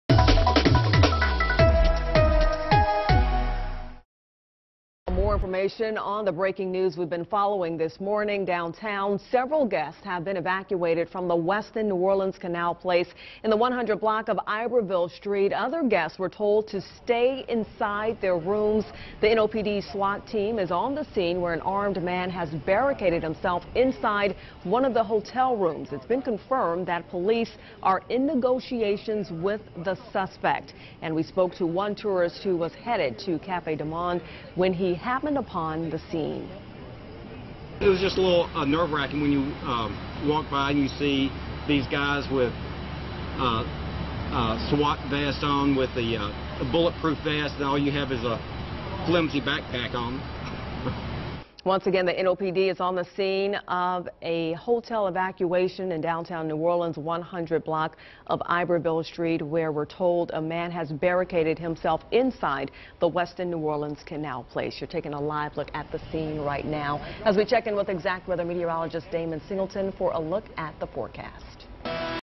Authorities in negotiations with armed man barricaded in downtown New Orleans hotel
New Orleans police said a man barricaded himself in a room on the 17th floor of the Westin Hotel on Sunday morning.